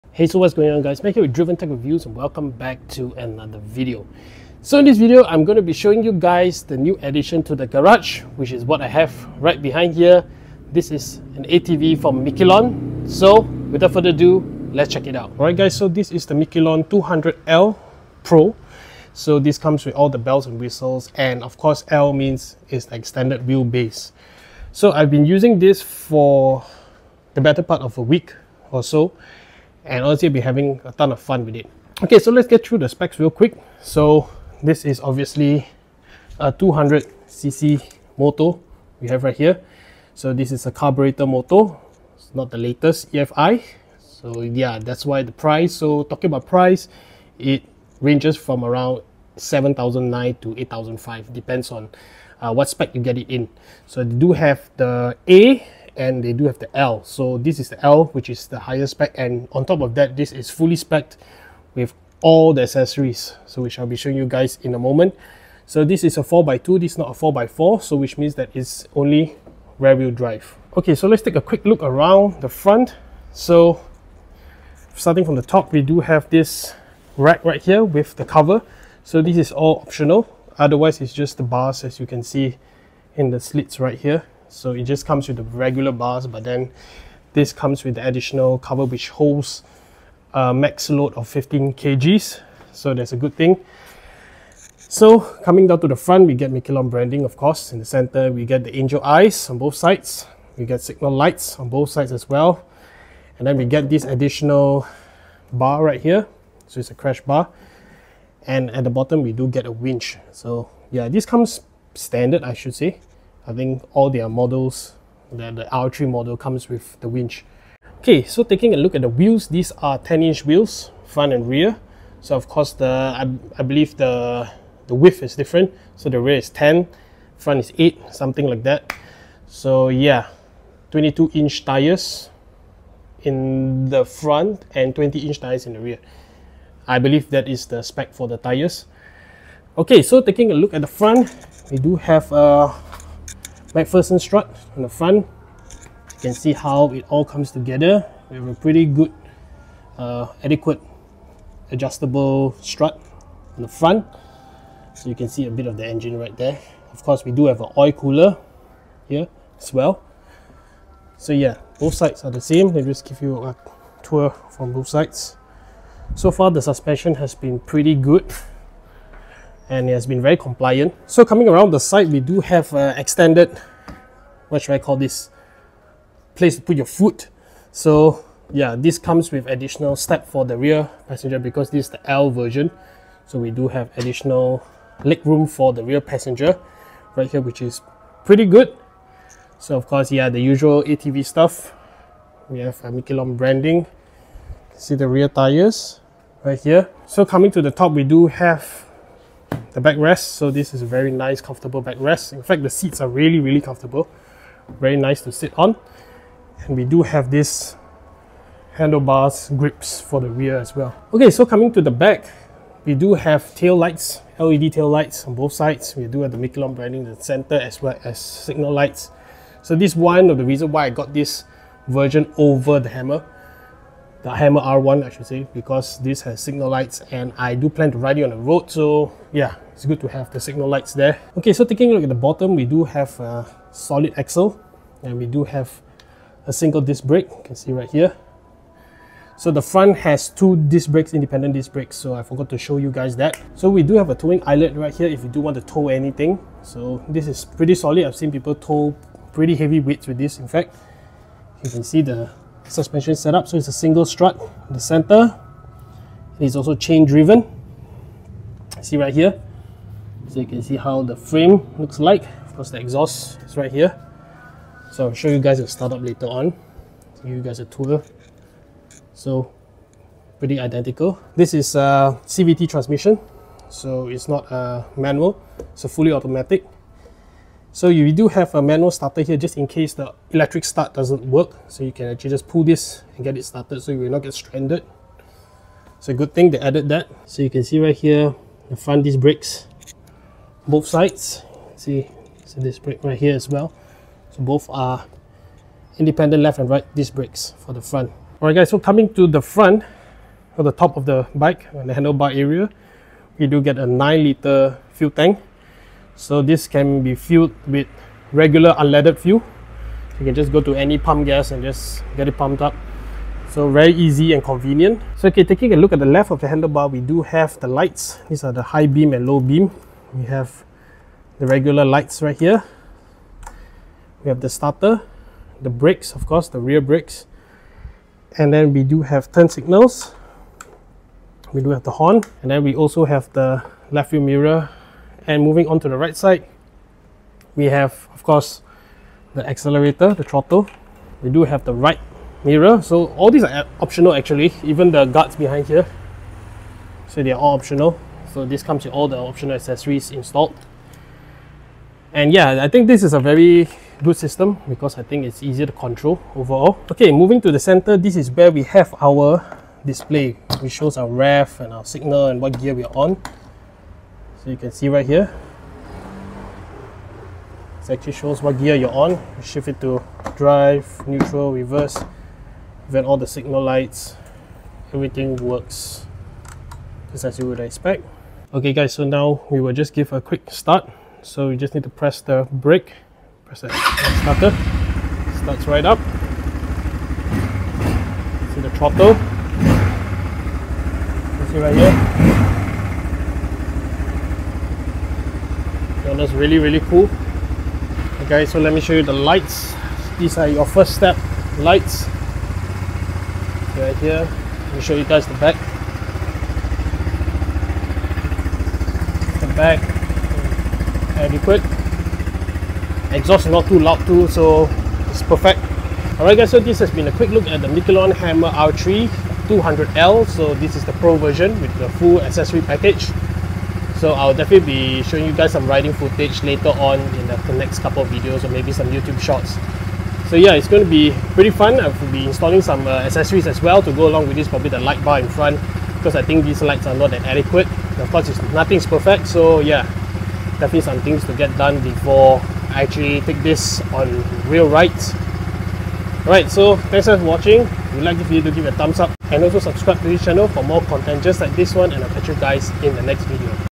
Mikilon Hammer R3 200L Quick Look
One of the most affordable and bang for buck ATVs around!

Follow Droid Tech on:
Instagram & Tiktok: @driventechreviews

Fliming Gear:
Insta360 GO
Insta360 One RS 1-Inch
RODE VideoMicro
Zoom H1